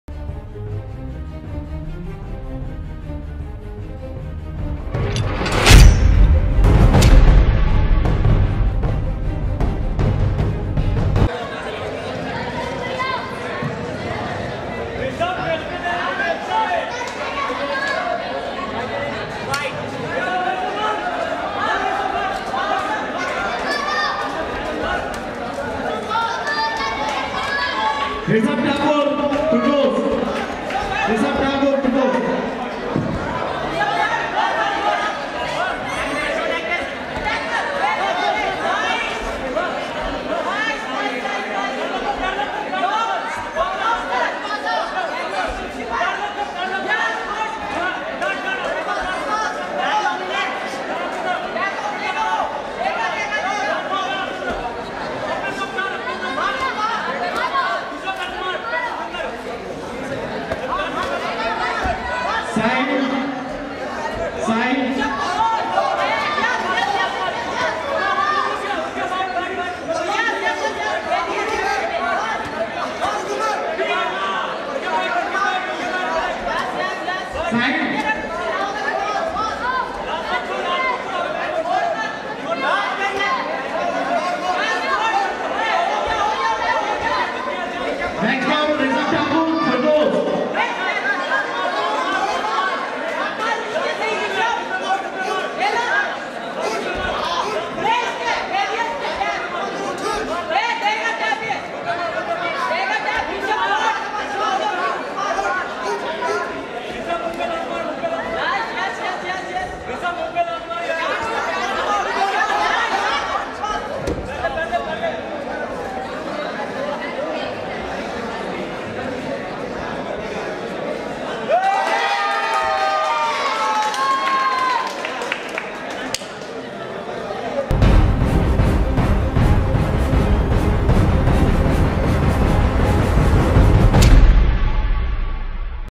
Knockout Hosharafu MMA #98
6th International MMA Championship 2018. Date 24 & 25 November 2018  at Janakpuri New Delhi, India.